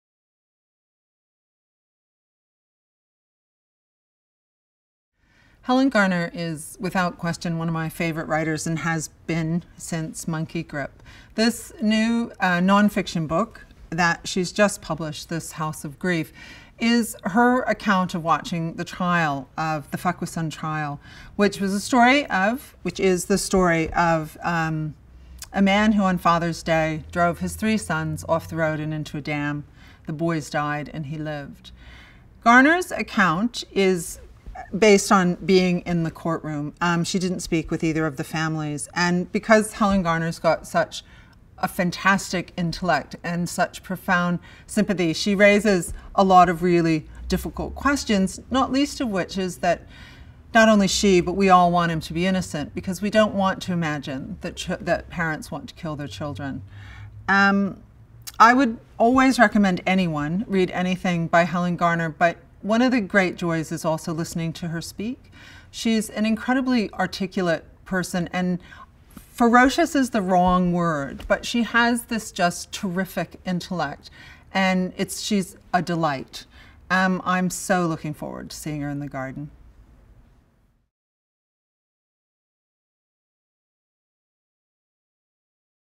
Helen Garner | Adelaide Writers' Week 2015
Adelaide Writers' Week 2015: Helen Garner

Adelaide Writers' Week director Laura Kroetsch introduces the Adelaide Writers' Week authors for 2015.

Cinematographer: Shane Reid
Editor: Chris Kellett
Sound Recordist: Lachlan Coles

Produced by Adelaide Festival

Special thanks to: State Library of South Australia